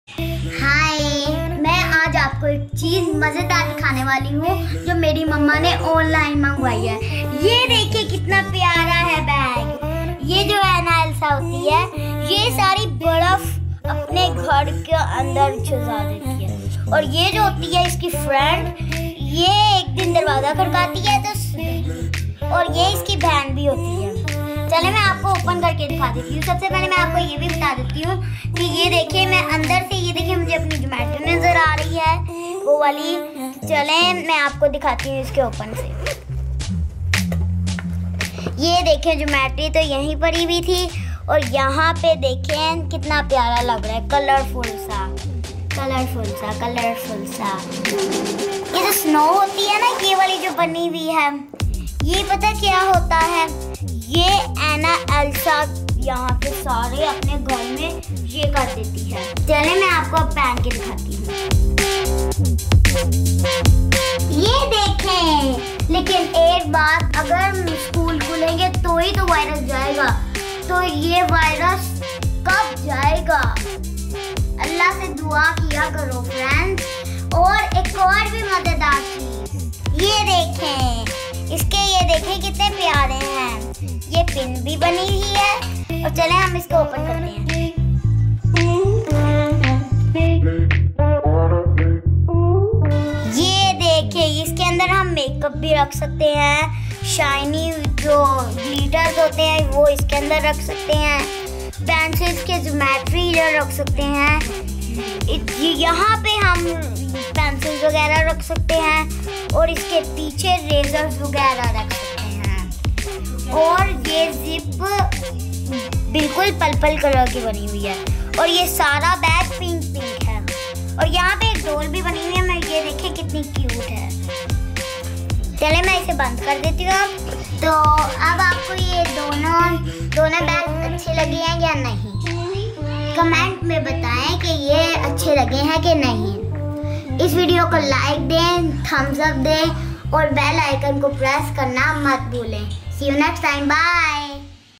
My Mom Ordered Me Bags Online | Yashfa Chishti
My mom ordered these cute bags for me online. I wished to show you all these bags. Take a look and see how you feel about these bags.